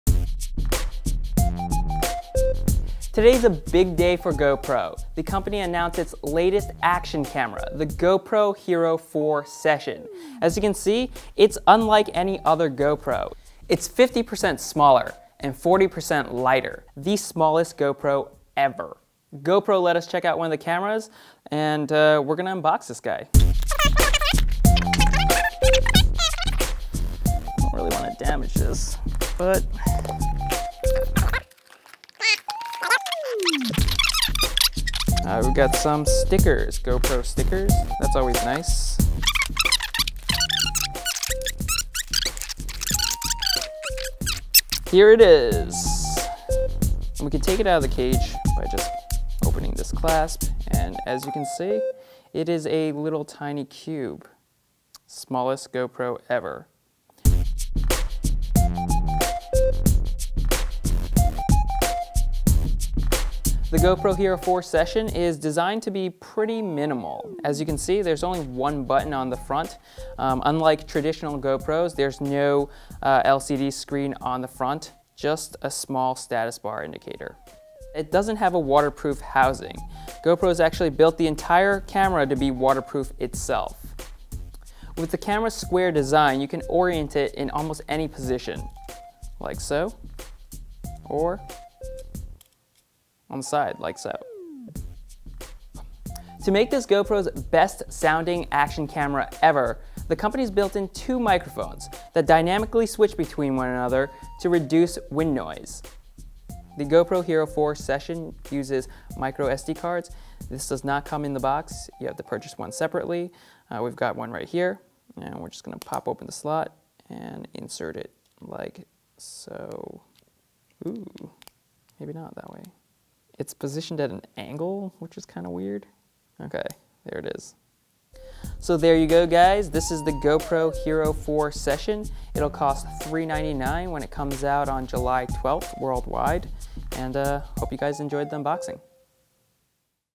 Unboxing the HERO4 Session, the smallest GoPro ever
Mashable's Product Analyst Raymond Wong gets his hands on GoPro's smallest and lightest action camera ever, the new HERO4 Session.